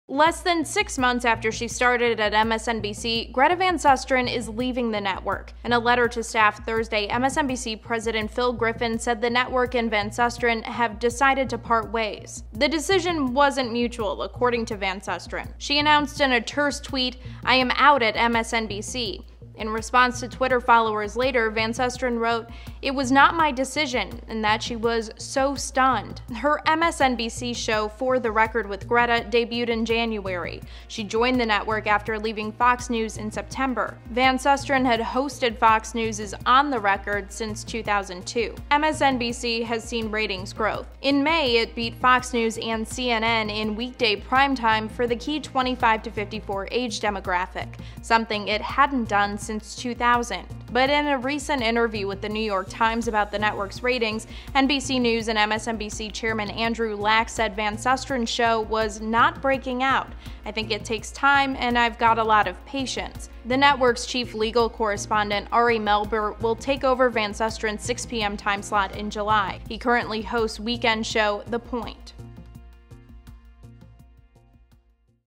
MSNBC parts ways with Greta Van Susteren
The Fox News alum joined MSNBC in January to host "For the Record With Greta" at 6 p.m.